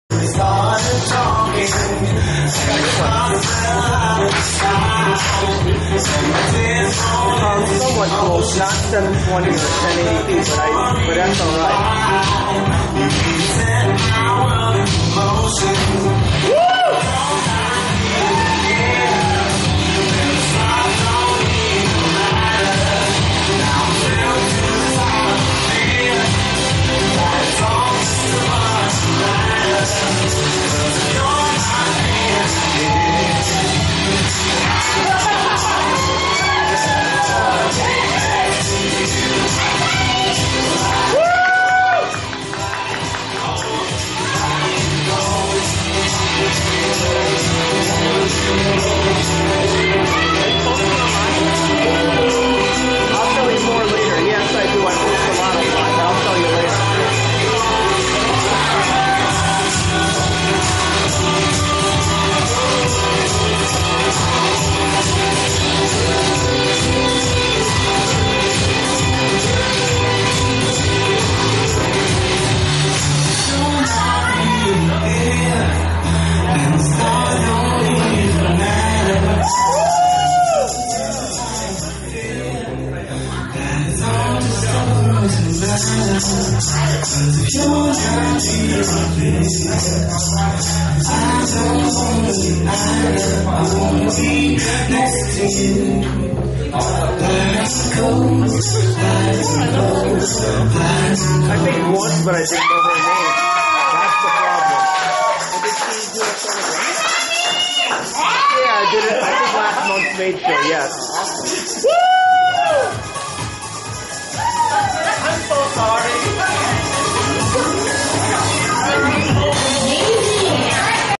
Reni Mimura's Maid Night (21+) 08-25-2011: Masami Ishibashi
Event: Reni Mimura's Japanese Maid Night (21+) at Radio Star Karaoke & Bar
Performer: Masami Ishibashi